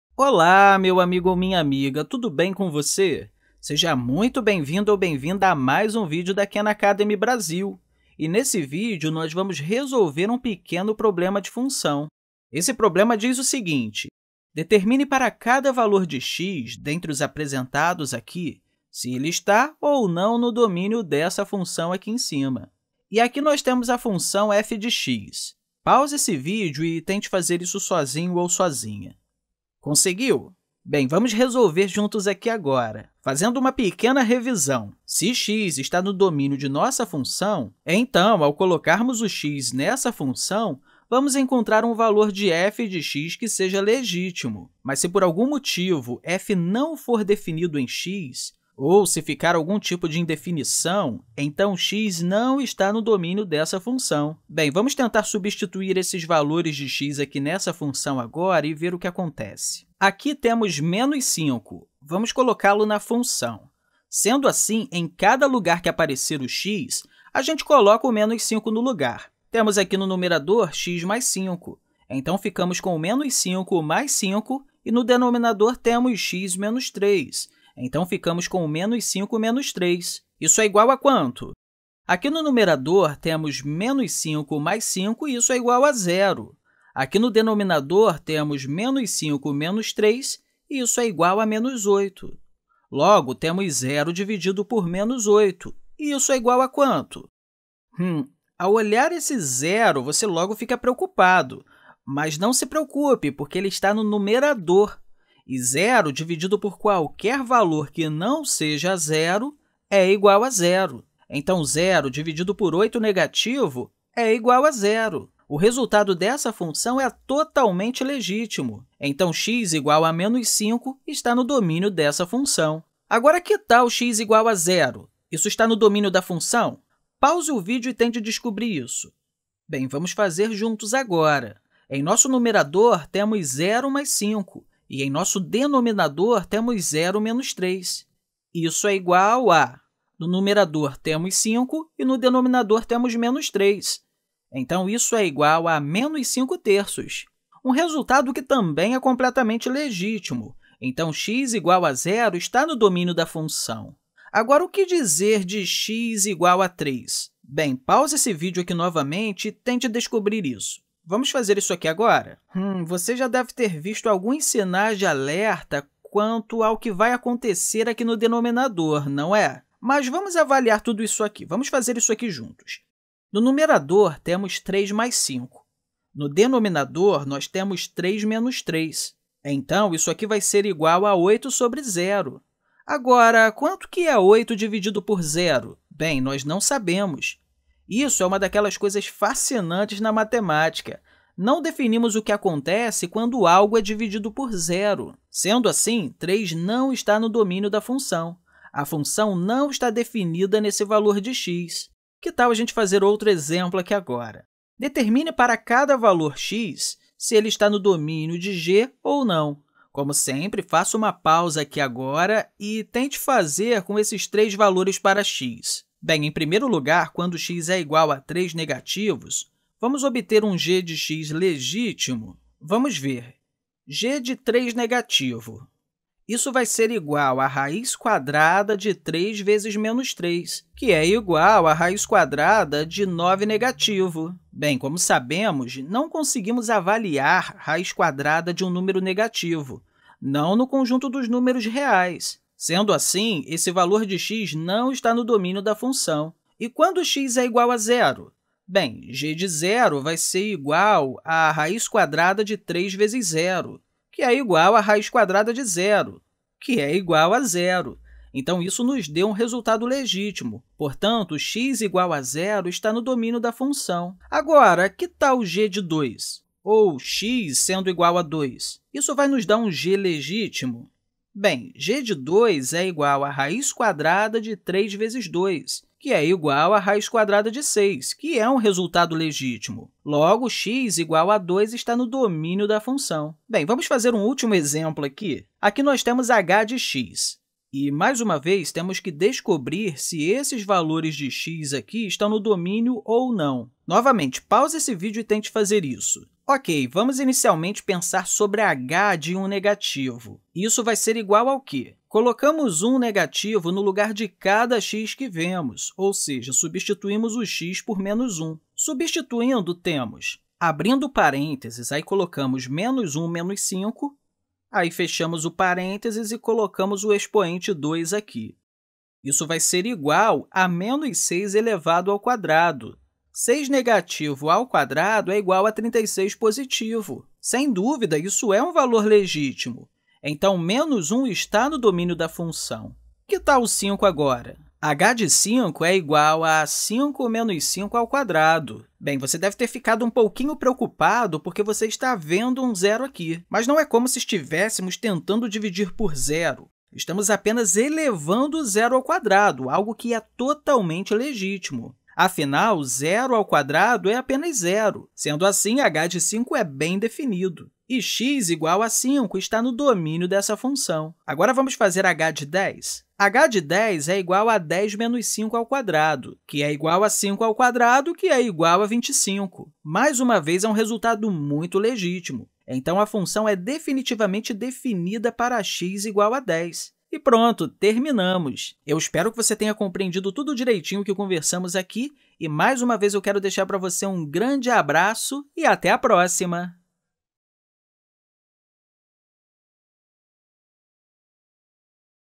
Determinando quais valores estão no domínio das funções
Nesse vídeo observamos algumas funções e determinamos se cada um dos valores apresentados faz parte do domínio da função.